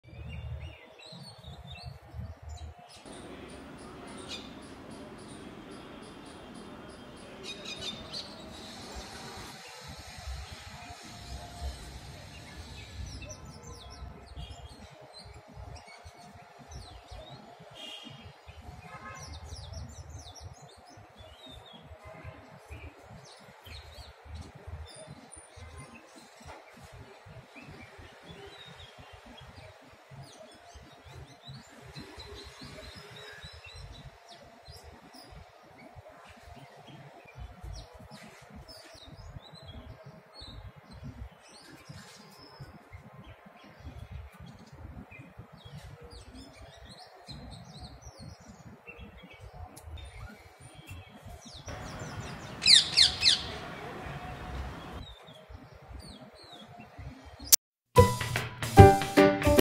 Tota-Maina (तोता-मैना) / Parrot- Indian Myna.
Tota- maina ki kahanai ( तोता-मैना की कहानी) is a famous story we all have listened it in our childhood. One day we got a chance to observe a parrot (मिठ्ठू तोता) and a pair of Indian Myna in our balcony.
Myna Pair was having its feed on the bird feeder. A parrot came down and got to the feeder. Myna left the place without any fight and this pair managed only to have scattered food.
These light moments  could be captured after lots of effort from our mobile camera.